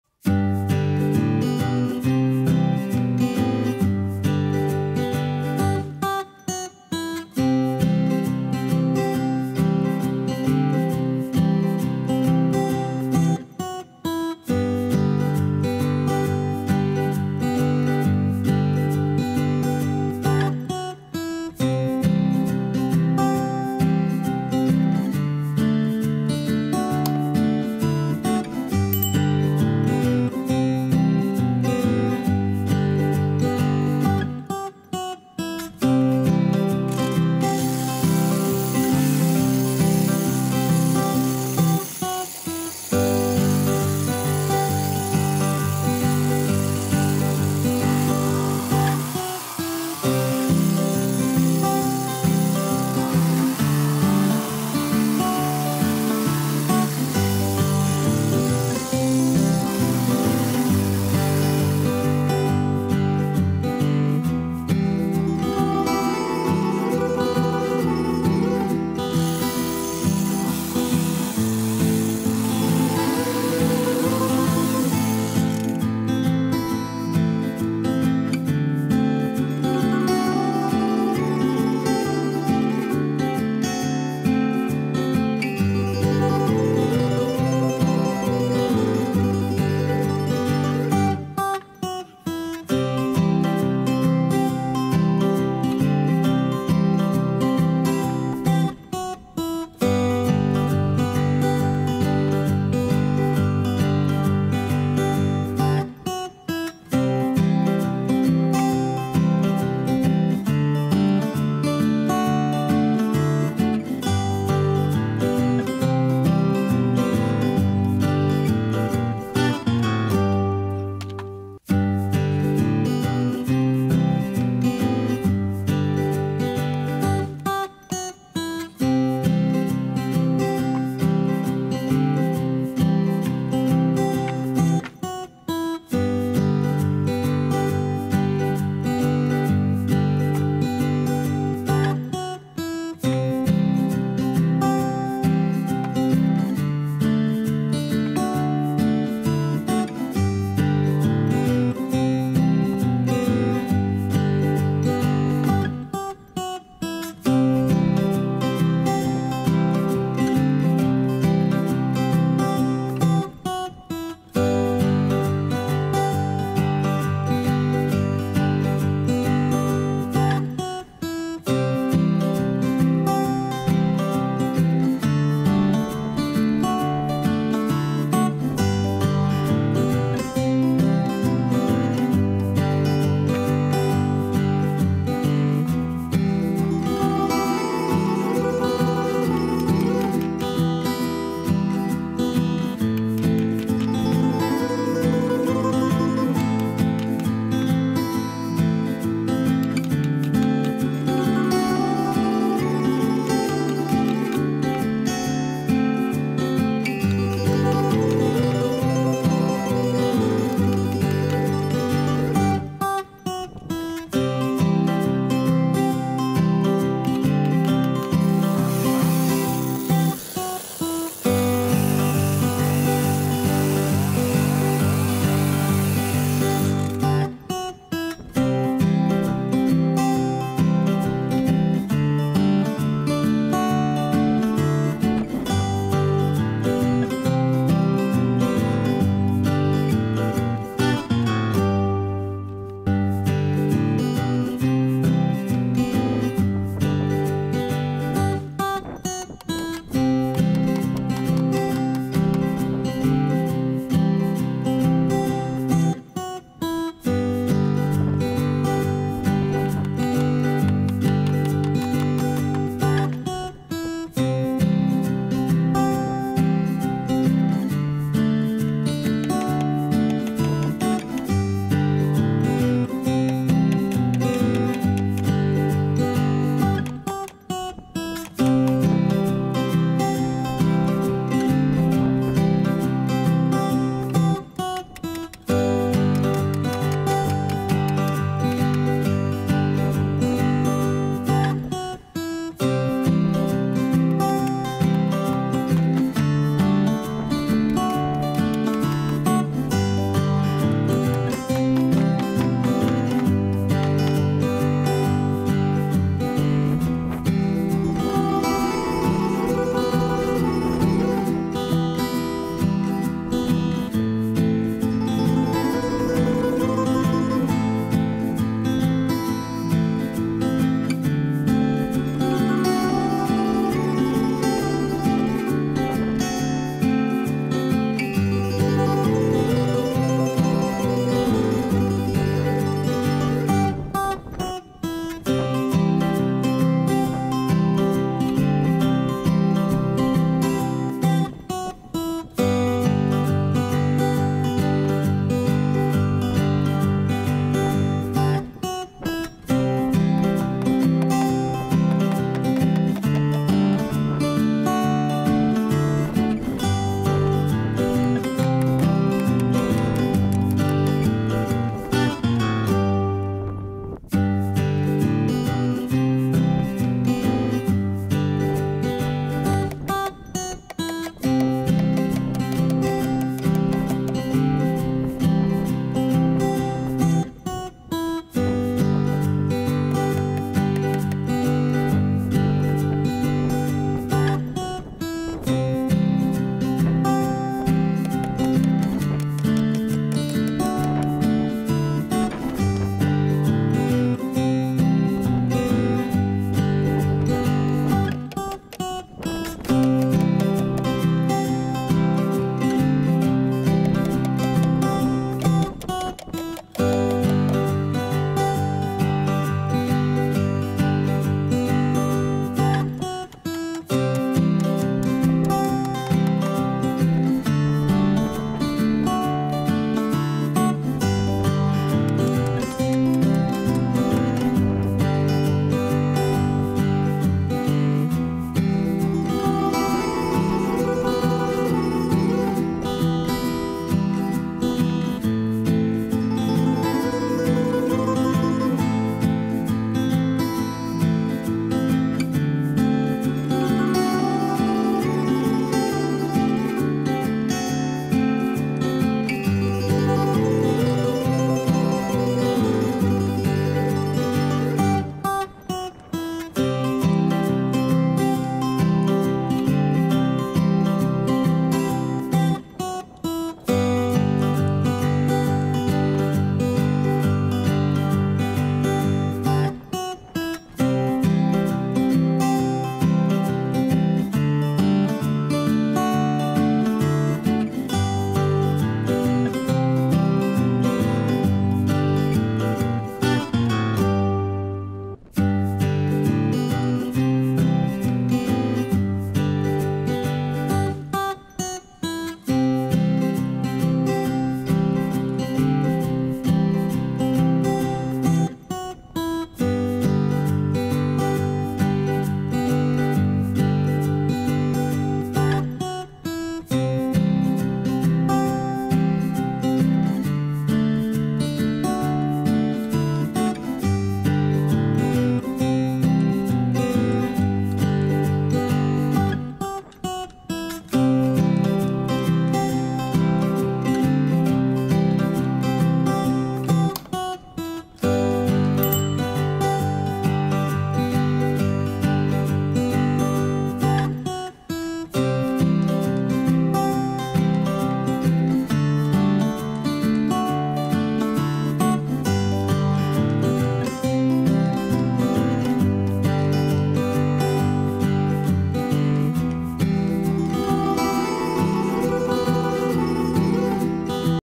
ДЕЛАЮ СКВОРЕЧНИК ДЛЯ ПТИЦ
Всем привет.Пришла весна.К нам в деревню прилетели скворцы.Птицам как и людям нужен дом.Сегодня я решил построить скворечник для скворцов.